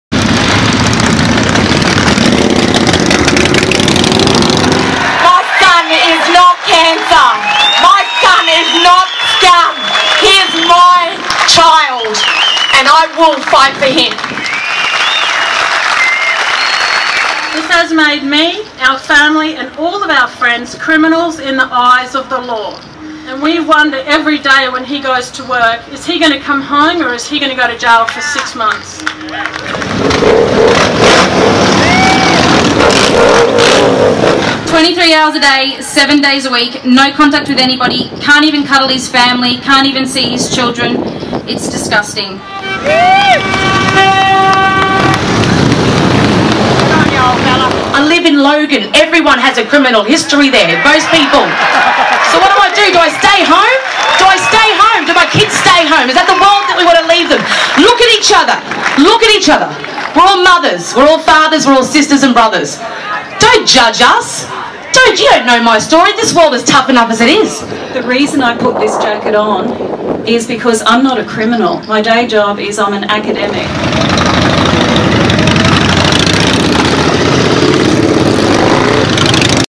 Women of the Australia Day Freedom Rally
Wives, mothers and daughters tell of the impact of the Queensland Government's anti-association laws. Stay in touch on this issue at MotorbikeWriter.com.